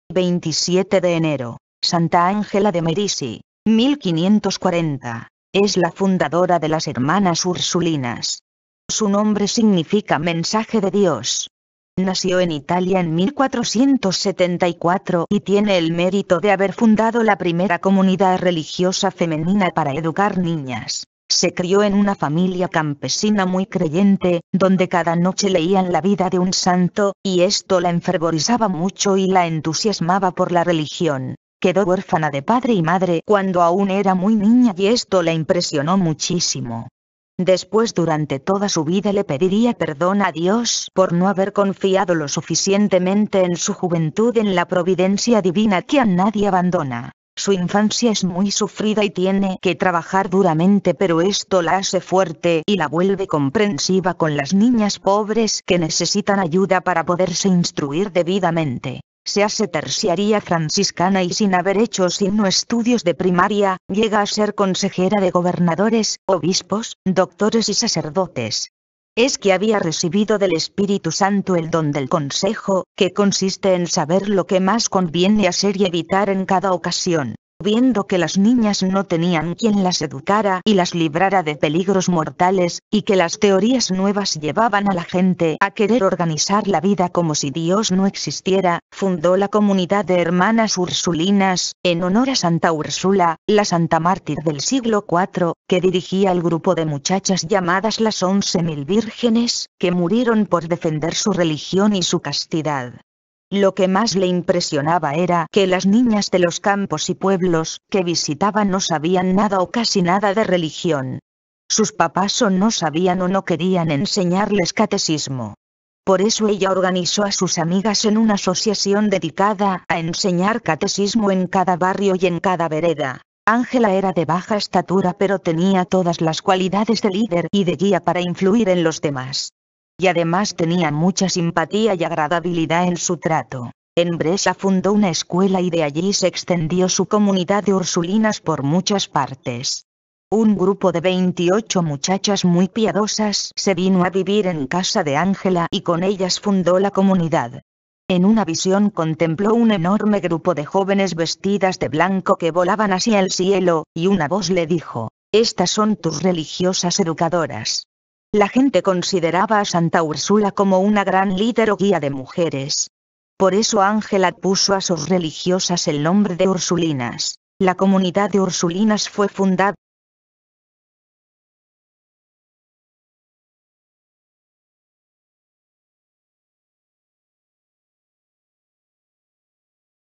Santa Angela de Merici,27 de Enero,Vidas Ejemplares
27 de Enero

Santa Angela de Merici
(+ 1540)

 Es la fundadora de las Hermanas Ursulinas. Su nombre significa "Mensaje de Dios".

Nació en Italia en 1474 y tiene el mérito de haber fundado la primera comunidad religiosa femenina para educar niñas.